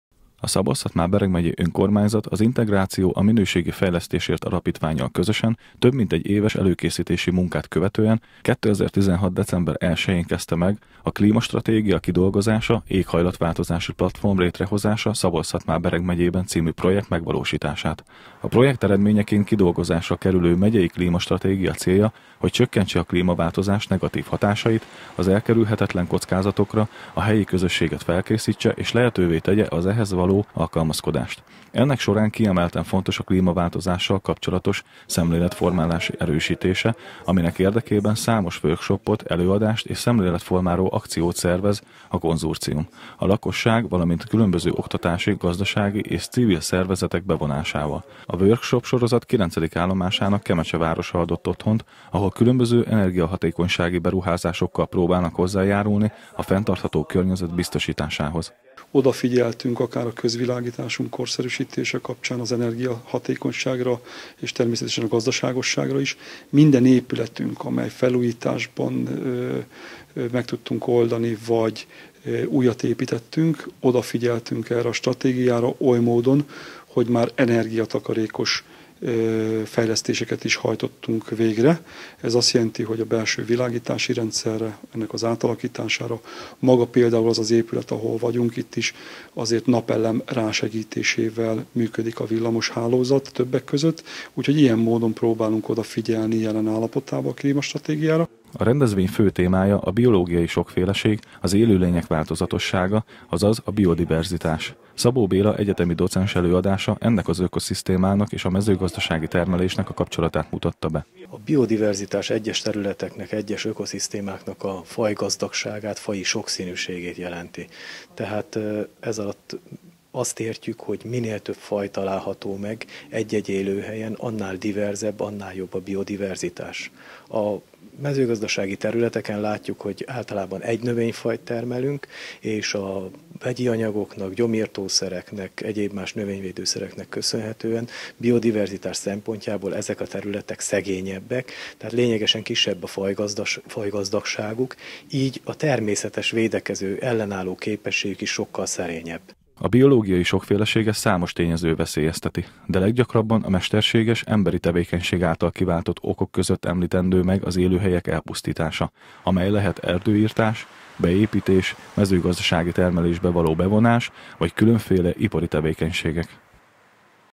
Fórum a klímaváltozásról - Kölcsey Tv
A biológiai sokféleség és a mezőgazdasági teremlés kapcsolatát mutatták be azon a rendezvényen Kemecsén, amely az éghajlatváltozás hatásaira hívta fel a figyelmet. A fórum egy, a megyei klímastratégia megalkotását segítő workshop-sorozat részét képezte.